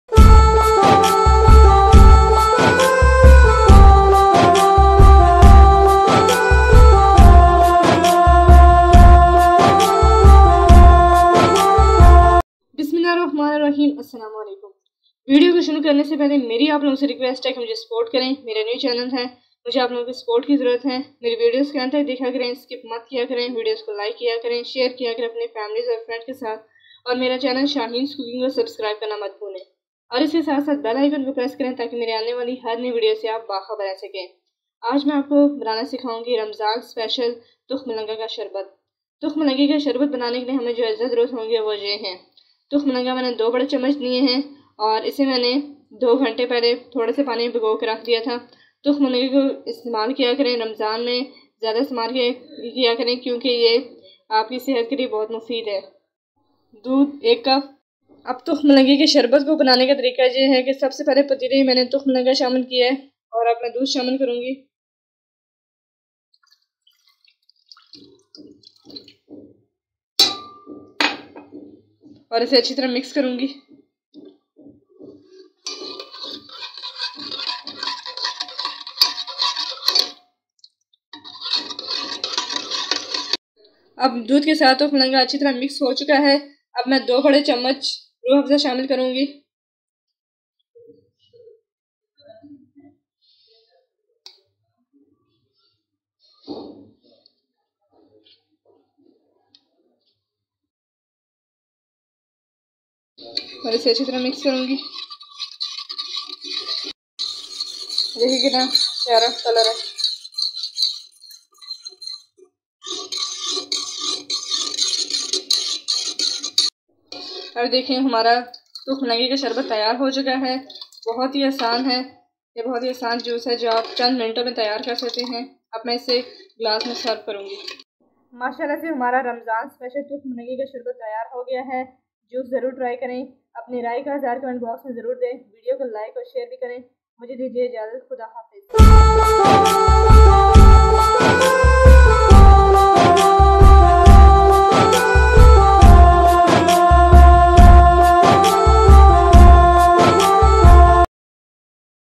Tukh Malanga Sharbat Recipe | Tukh Malanga Drink | Ramzan Special Drink | Iftar Spe Cial Drink 2021
TRY THIS AT HOME AND SHARE WITH FAMILY AND FRIENDS